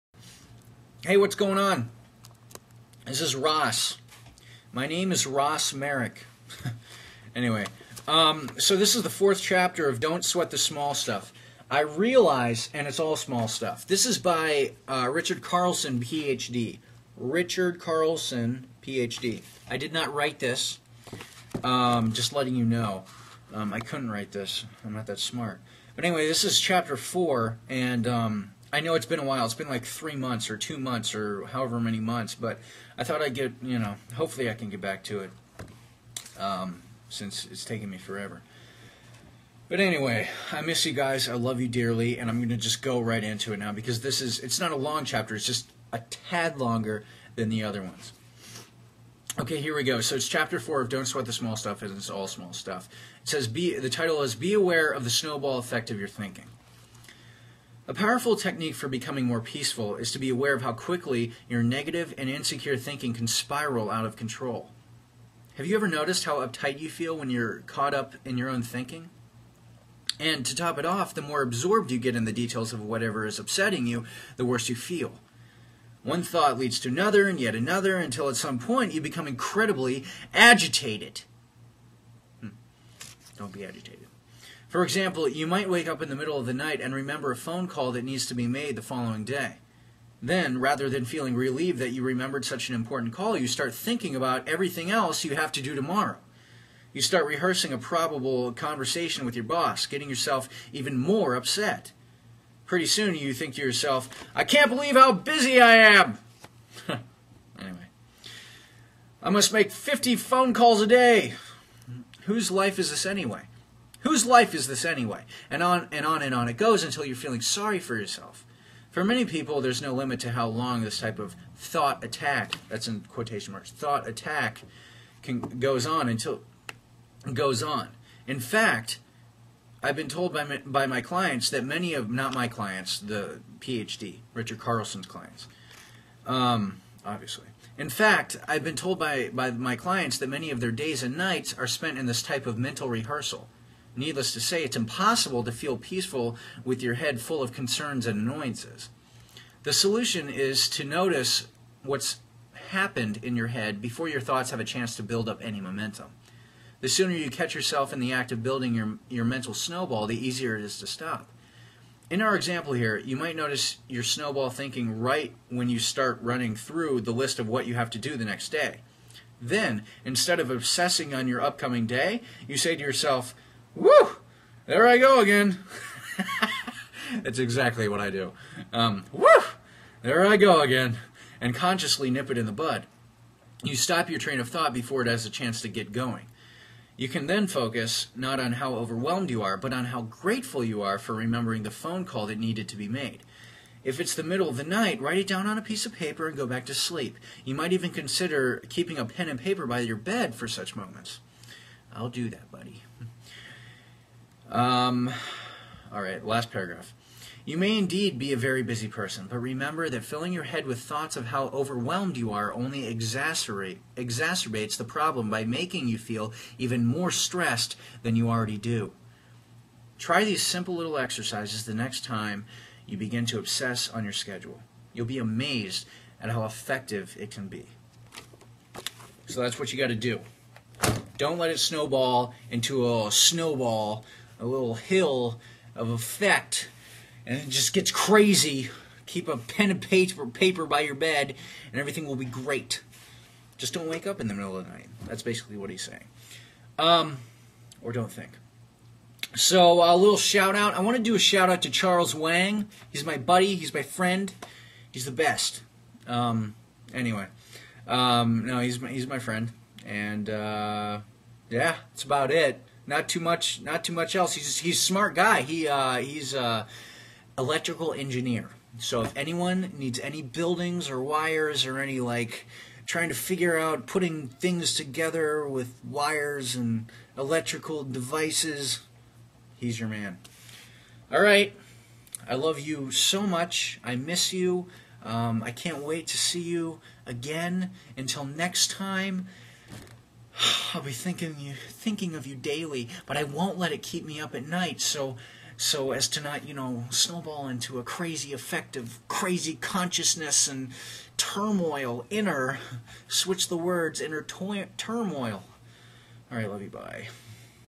Don't Sweat The Small Stuff Ch. 4
Dr Richard Carlson's book about simple ways to keep the little things from taking over your life. This Chapter is entitled "Be Aware of the Snowball Effect of Your Thinking."